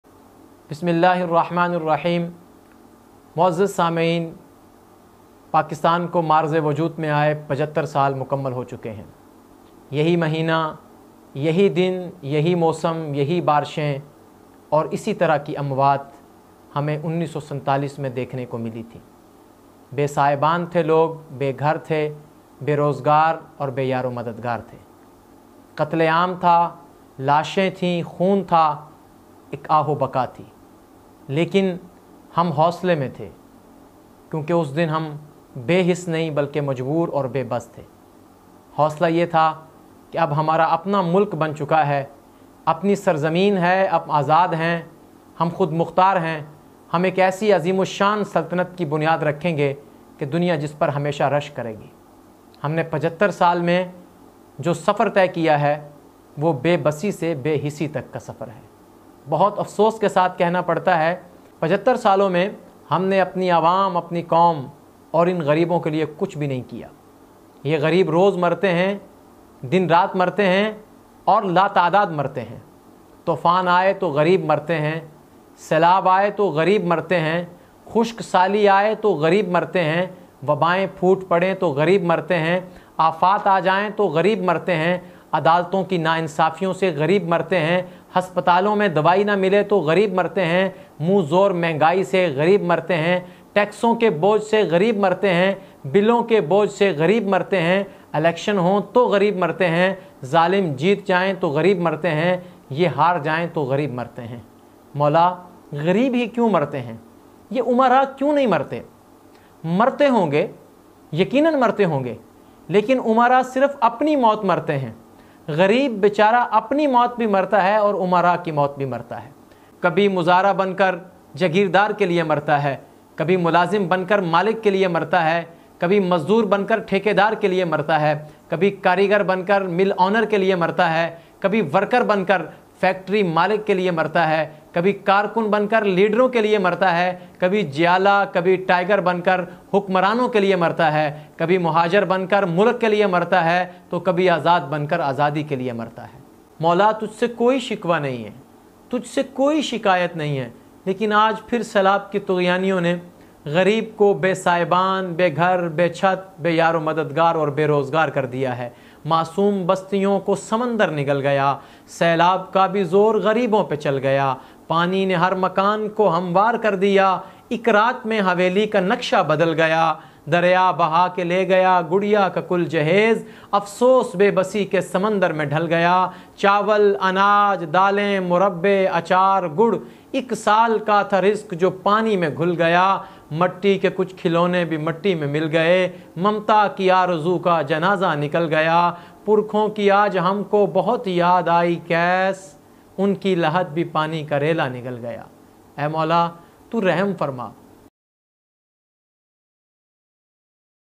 Greeb hi Kino Mrta h?By Dr M Faisal Azadغریب ہی کیوں مرتا ہے
Greeb hi Kino Mrta h?By Dr M Faisal Azadغریب ہی کیوں مرتا ہے Faisal Azad This is the official Youtube channel of Muhammad Faisal Azad .Faisal Azad is an Islamic Scholar, Motivational Speaker and Trainer